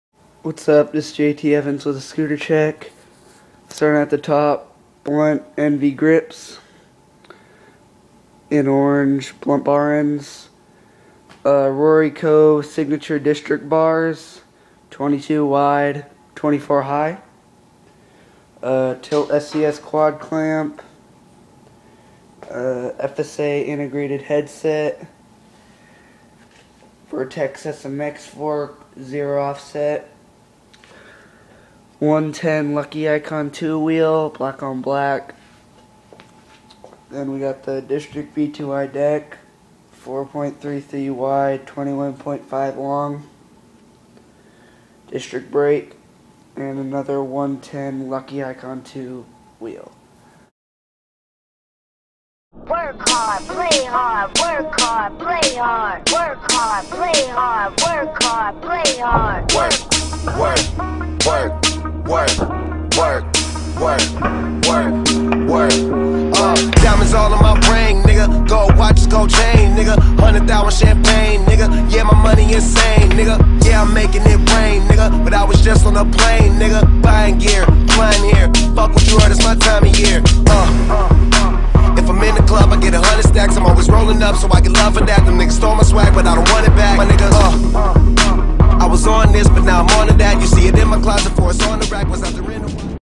Scooter Check with Clips
My Setup and some clips. I know the clips sucked but i didnt have the right stuff to trick on. My summer edit will be done and up by august 27th!:D